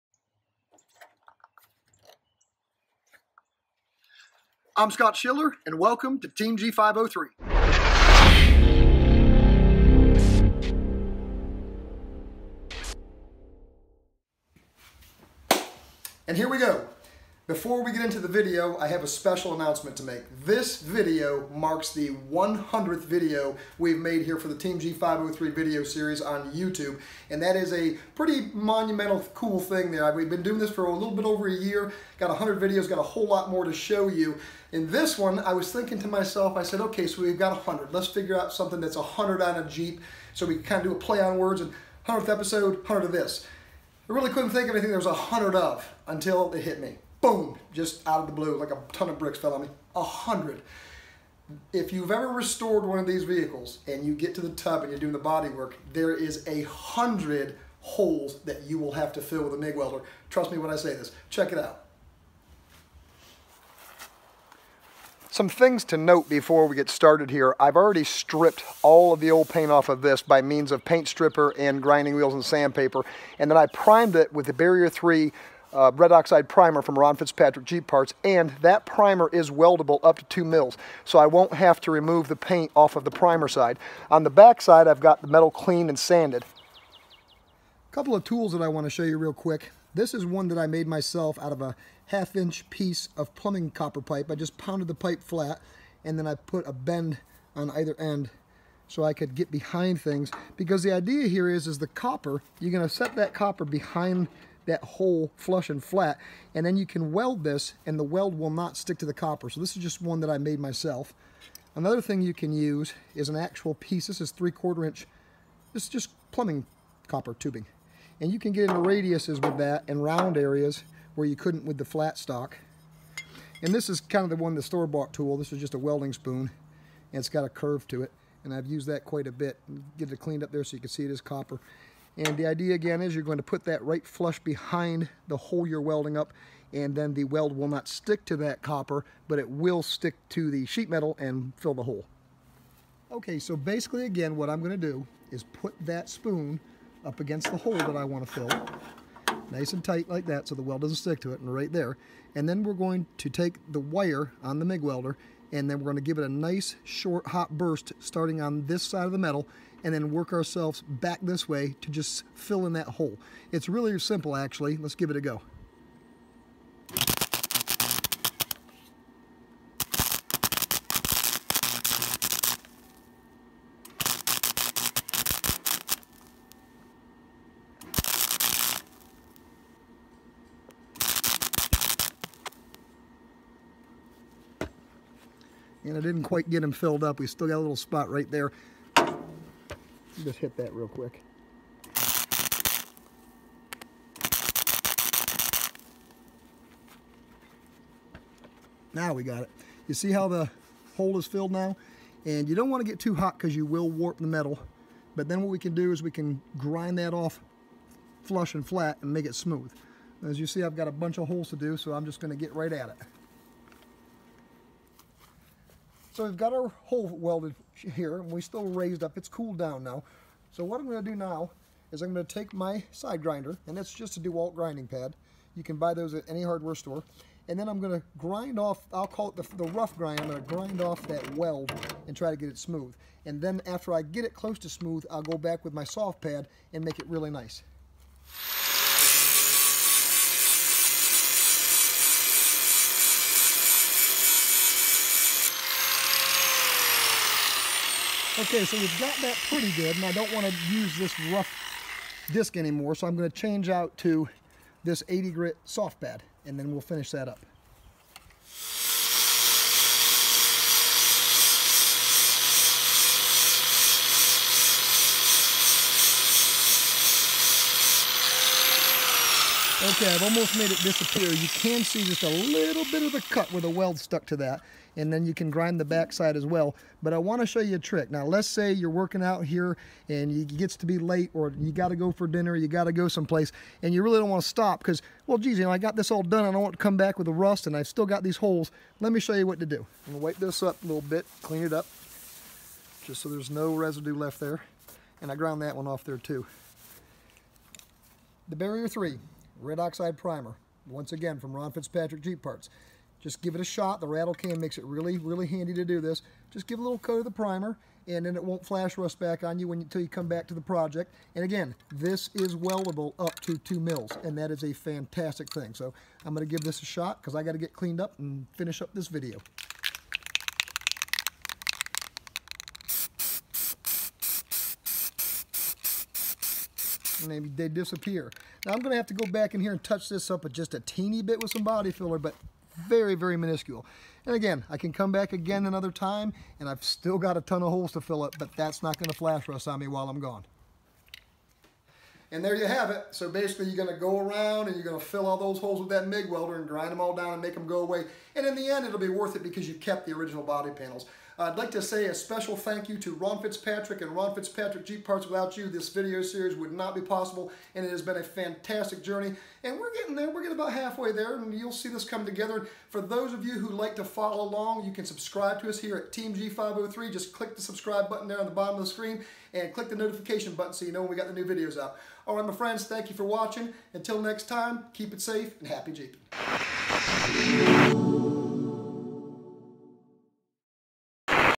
Willys MB Team G503 100th Video Welding Holes In Sheet Metal
100th video in the Team G503 Video Series on the restoration of a 1943 Wiilys MB. All parts provided by Ron Fitzpatrick Jeep Parts.